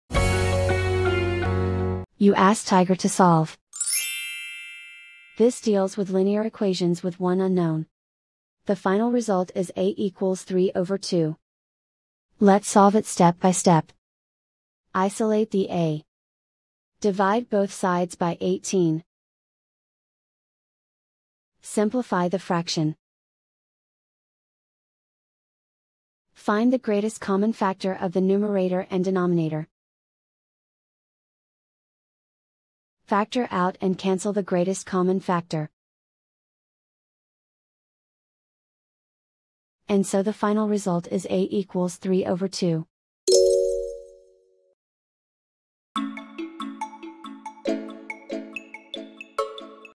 Linear equation with one unknown: Solve 18a=27 step-by-step solution
Solving 18a=27 using the linear equation with one unknown solver

for a step-by-step and more tips and tricks for dealing with linear equations with one unknown.